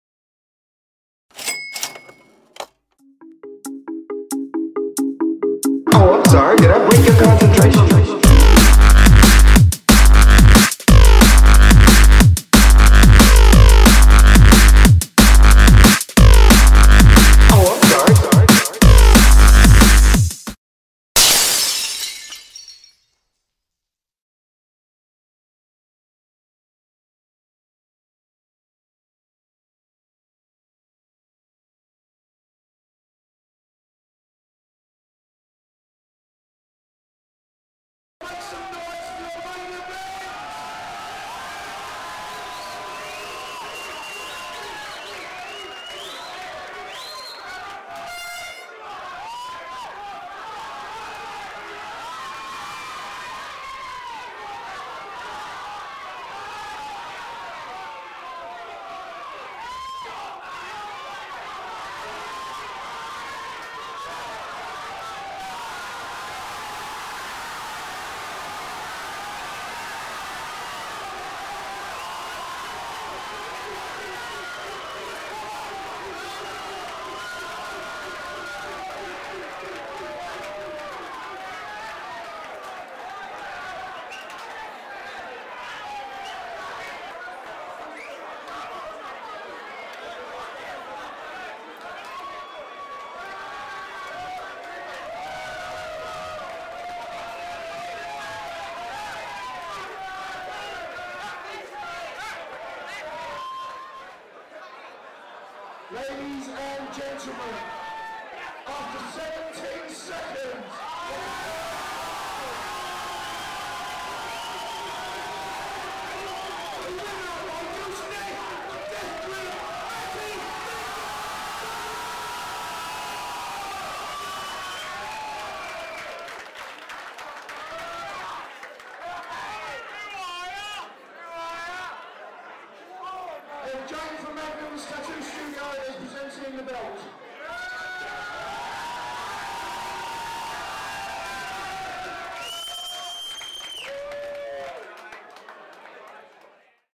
BCMMA3 - 11/05/13 Nikki Kent Vs. Matty BigDawg Clarke - British Pro Heavyweight Title Contest
BCMMA3 - The CashGenie British Challenge

Watch two heavyweights go toe to toe for the BCMMA Pro Heavyweight Title.

Filming by ReevesTrademark Productions.